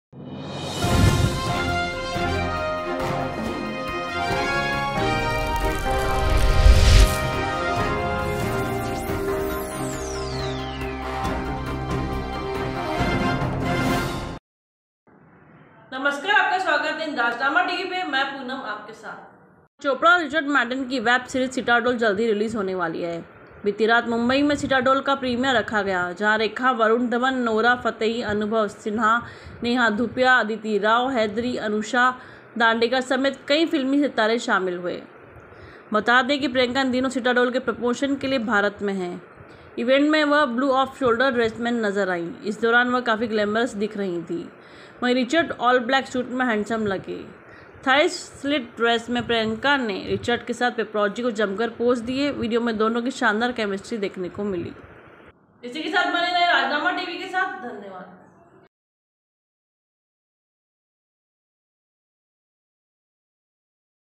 anchorpoonam प्रियंका चोपड़ा की सीरीज 'सिटाडेल' के प्रीमियर में पहुंचे कई सेलेब्स
About this video: प्रियंका चोपड़ा की सीरीज 'सिटाडेल' के प्रीमियर में पहुंचे कई सेलेब्स